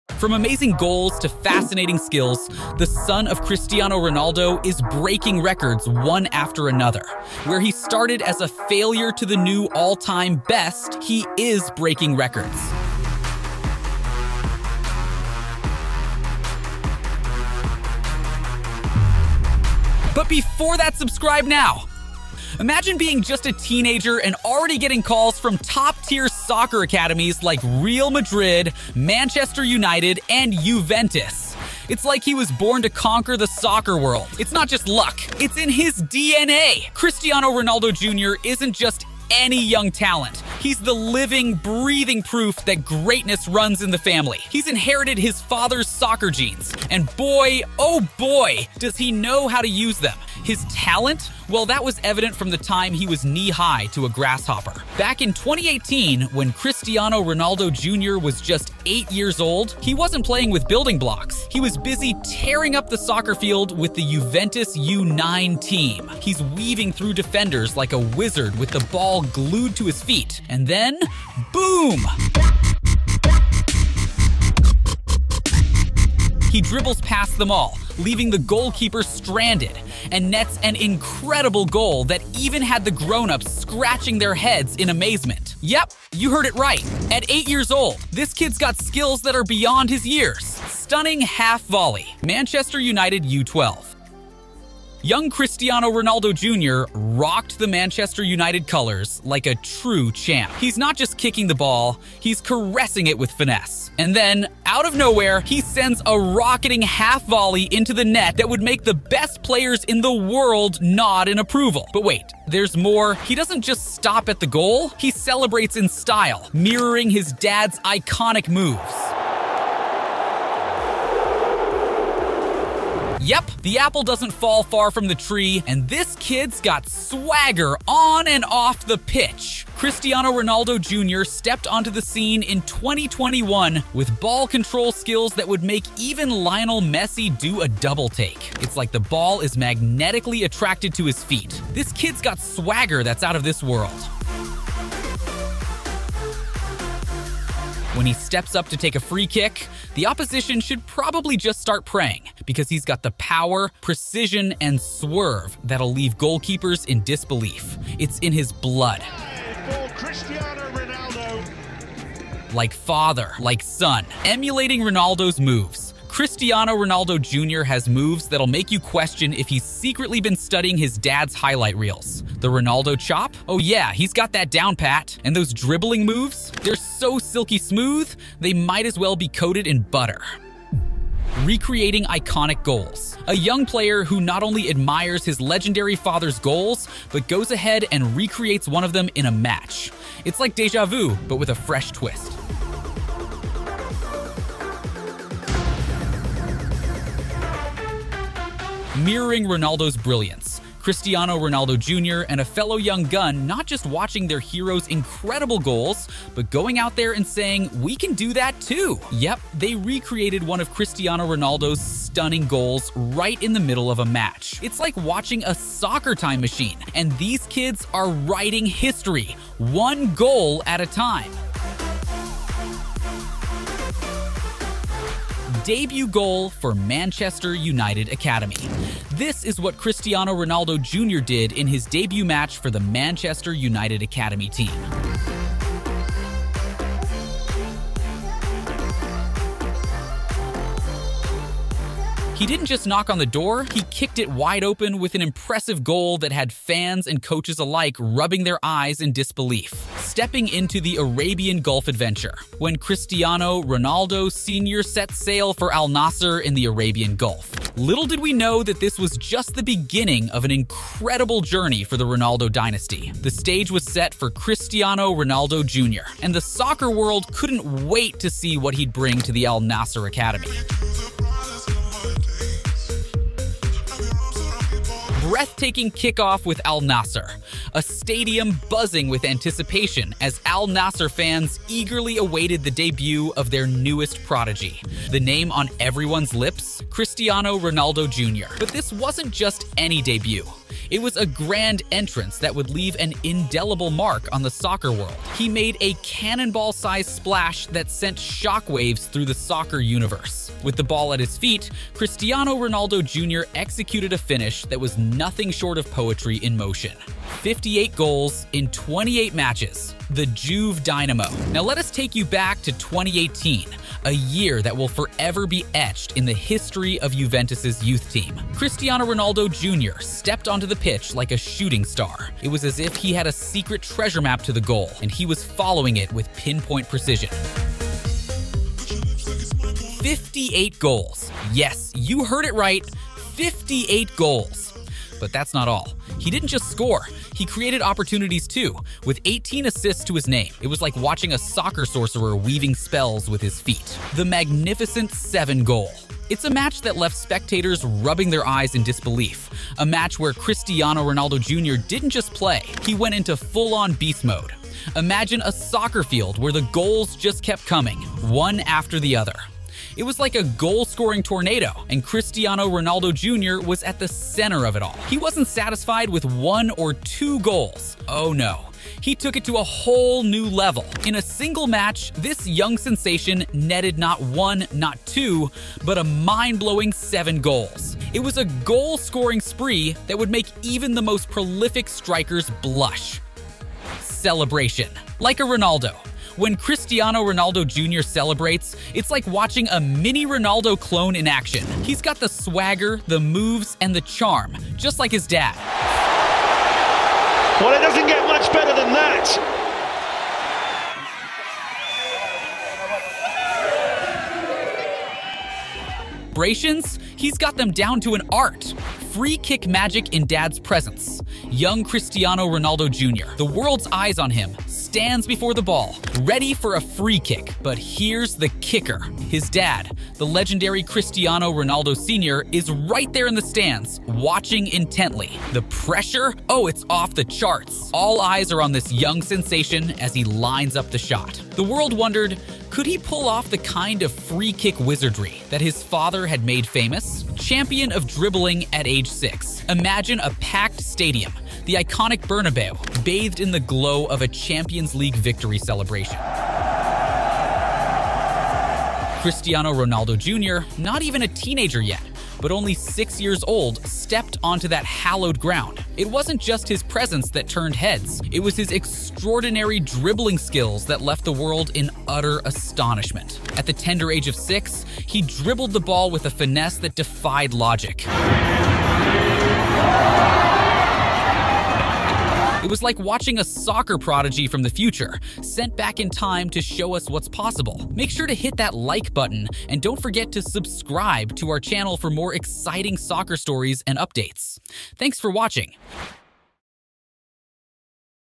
15 Times Ronaldo Jr Masterclass Shocked The World ...
15 Times Ronaldo Jr Masterclass Shocked The World ...

Ronaldo Junior, despite his youth as a teenager, is displaying the makings of a future legend! Throughout his budding career, he has graced some of the finest football academies worldwide, including those of Real Madrid, Manchester United, and Juventus.

Undoubtedly, he's inherited Cristiano Ronaldo's exceptional talent, and his remarkable abilities and goal-scoring prowess are unequivocally showcasing this to the world. However, it's his inaugural goal that might stand as his most remarkable achievement yet...

➡️ For more updates and content like this we would really appreciate if you could:

💻 Links 💻
➜ NIKE Jersey
➜ FREE 6 Months AMZN PRIME